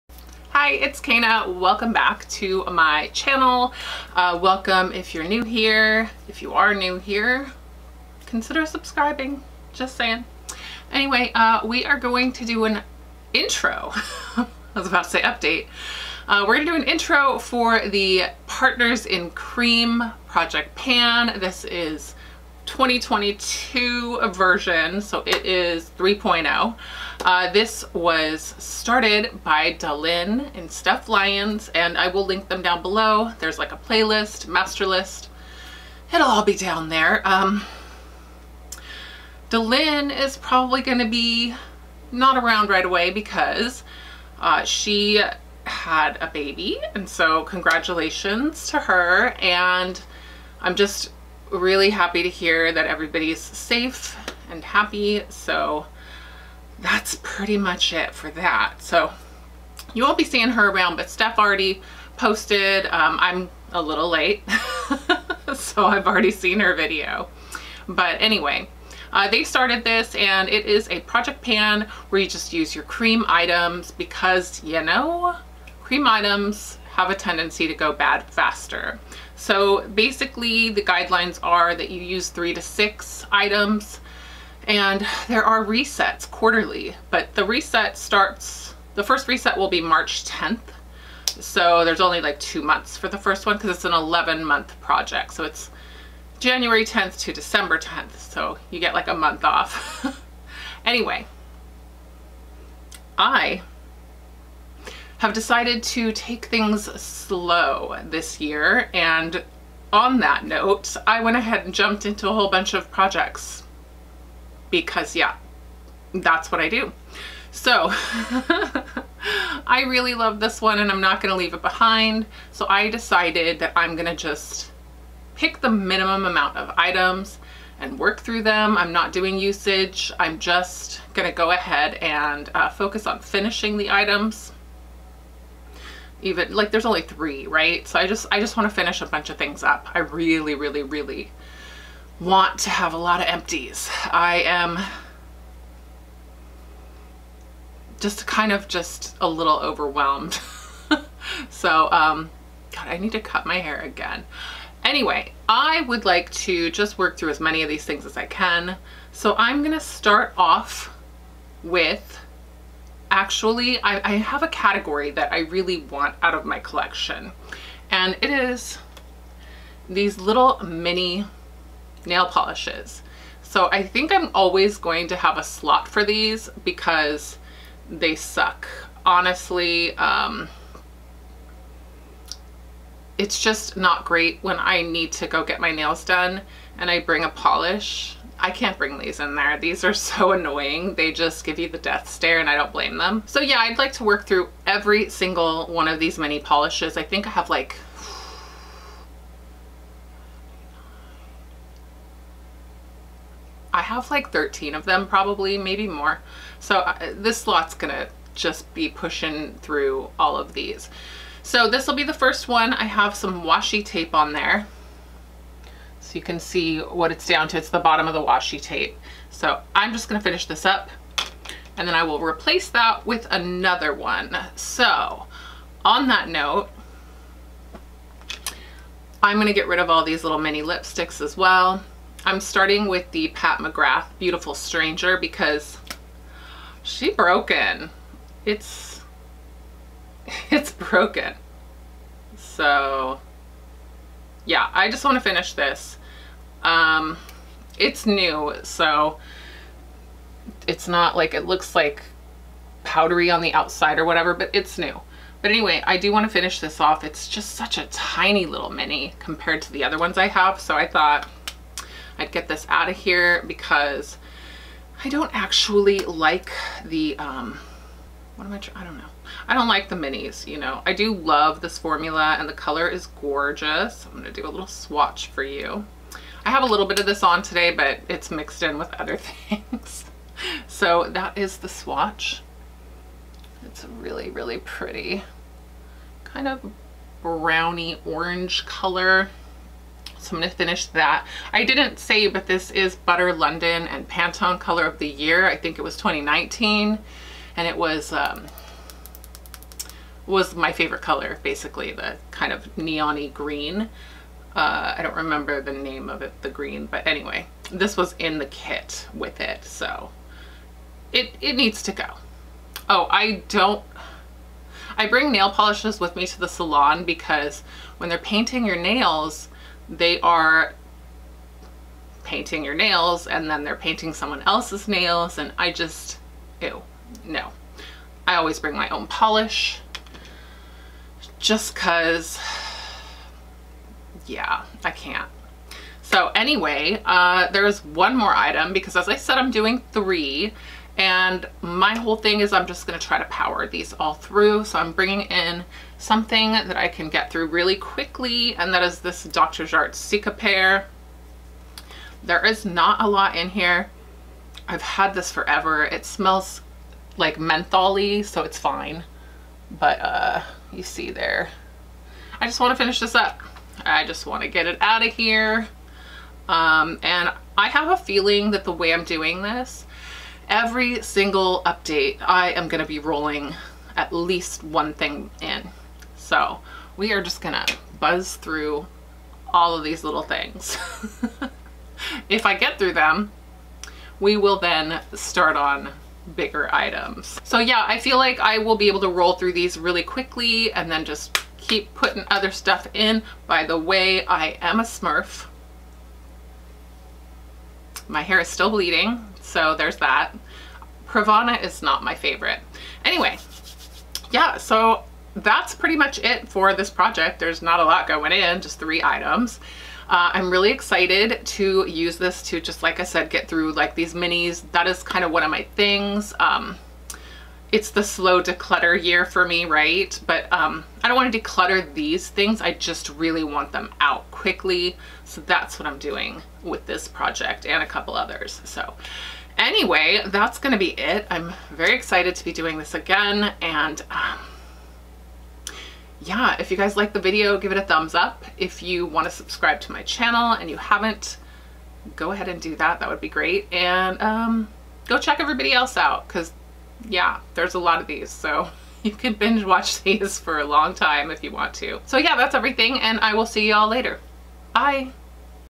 PartnersInCream2022 [INTRO!] | Using Up Cream Products BEFORE They Go Bad!
Partners in Cream is a panning project for creams, liquids... anything not in powder form, basically.

Project Information:
Intro:January 10th
Quarterly Refresh
3-6 Products
Rolling Style
Year Long
Updates 10th of Every Month
Monthly Updates

NEW 2022 SIGN UP SHEET - PLEASE ADD YOUSELF TO IT